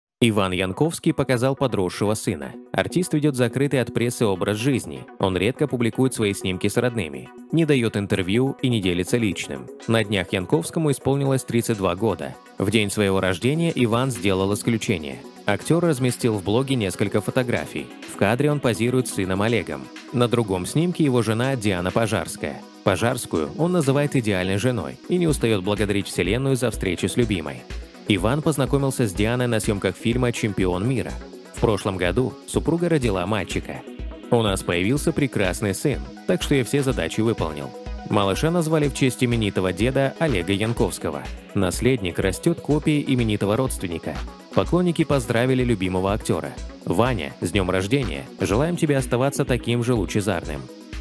Иван Янковский показал подросшего сына
Артист ведет закрытый от прессы образ жизни. Он редко публикует свои снимки с родными. Не дает интервью и не делится личным.
На днях Янковскому исполнилось 32 года. В день своего рождения Иван сделал исключение. Актер разместил в блоге несколько фотографий. В кадре он позирует с сыном Олегом. На другом снимке — его жена Диана Пожарская